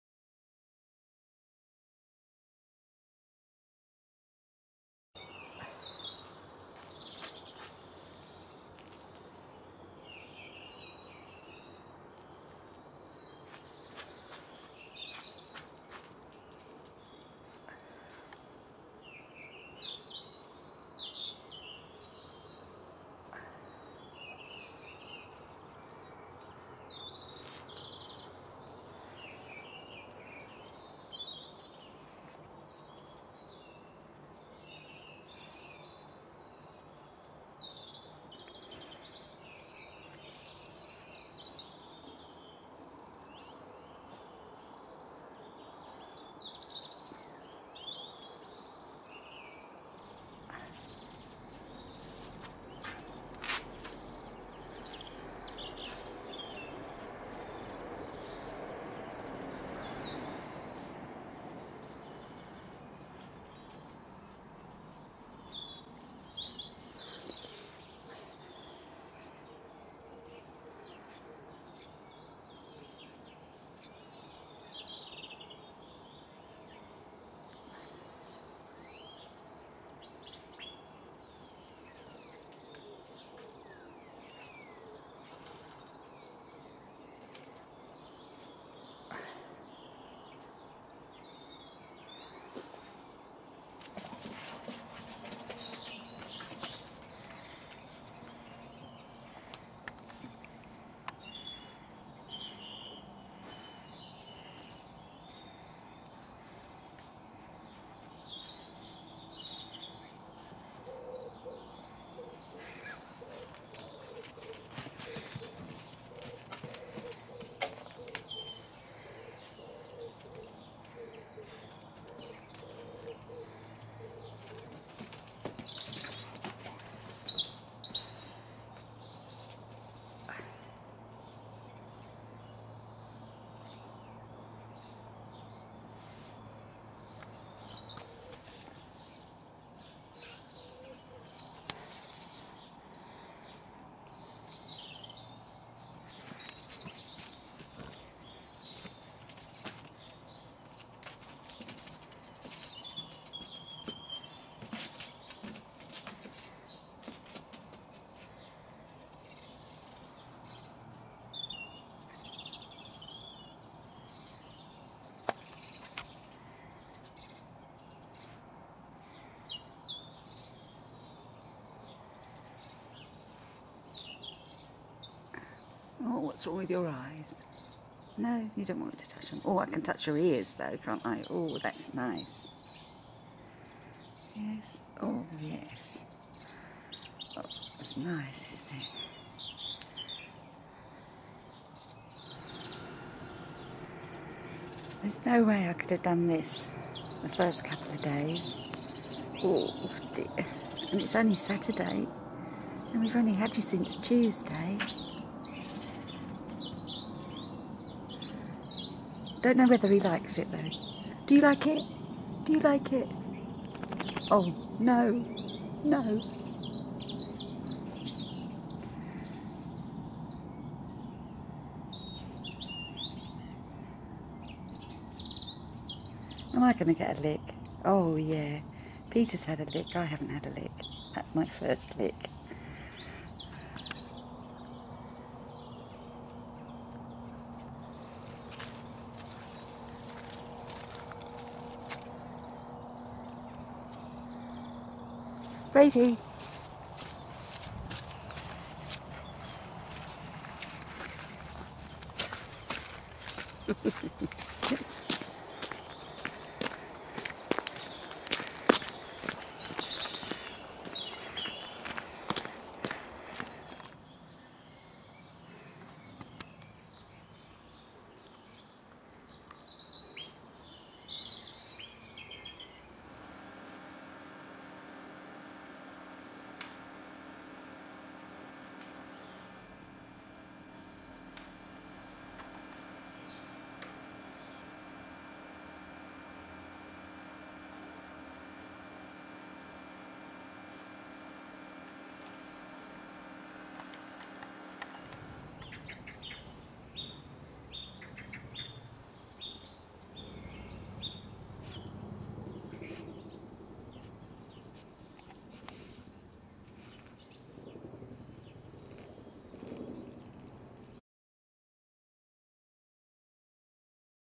Skiathos Dog Shelter - Braveheart - first few days.
Images of Greece - Braveheart from the Skiathos Dog Shelter - first few days at home in the UK.